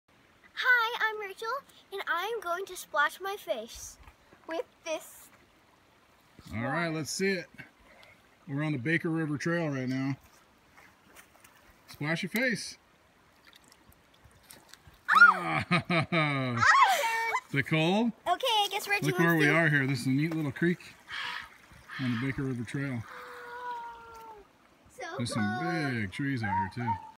Splash Your Face in a Creek When Hiking
Raichel loves to splash her face after a few miles on a hot hike. It is refreshing to say there least!